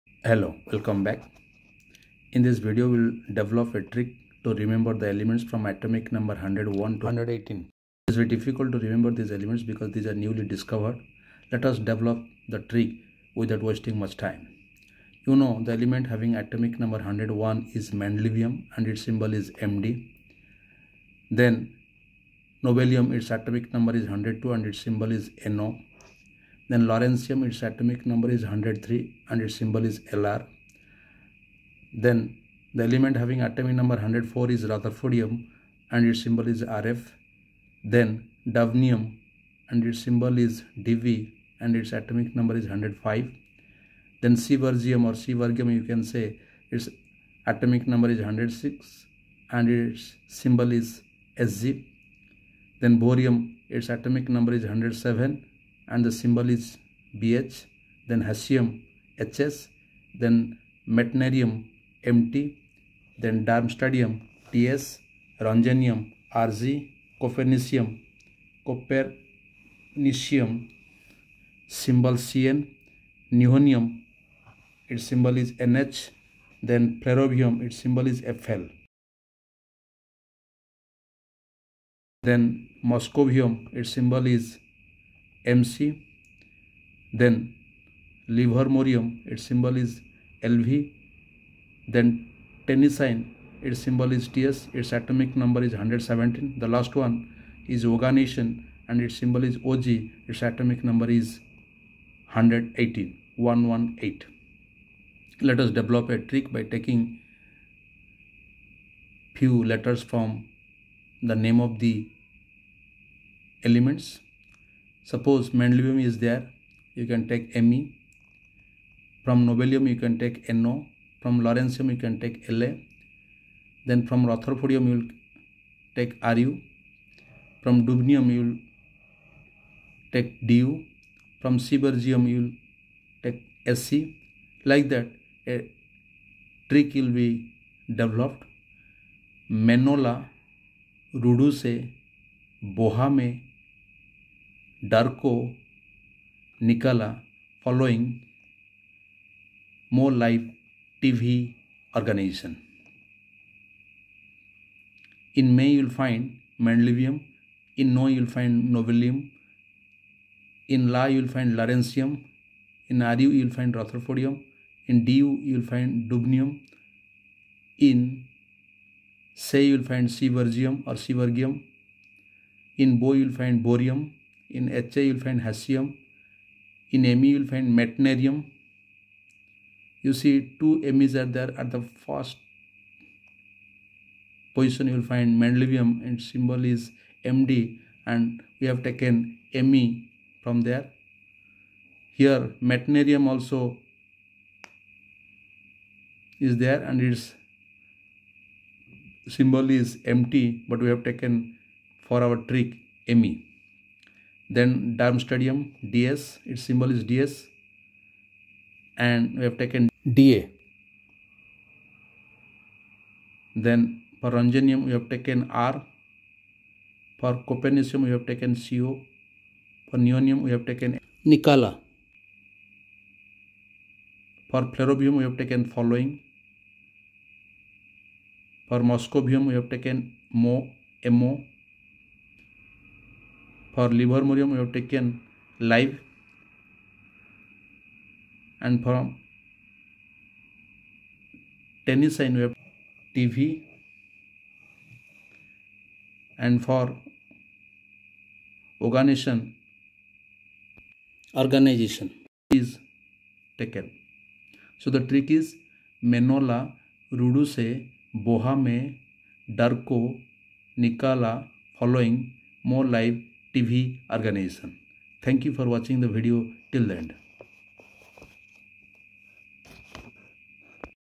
Trick to remember elements from atomic number 101 to 118/
Important  for NEET exam.
Mendelevium, Nobelium, Lawrencium, Rutherfordium, Dubnium, Seaborgium, Bohrium, Hassium, Meitnerium, Darmstadtium, Rontgenium, Copernicium, Nihonium, Flerovium, Moscovium, Livermorium, Tennessine, Oganesson.
Symbols of these elements.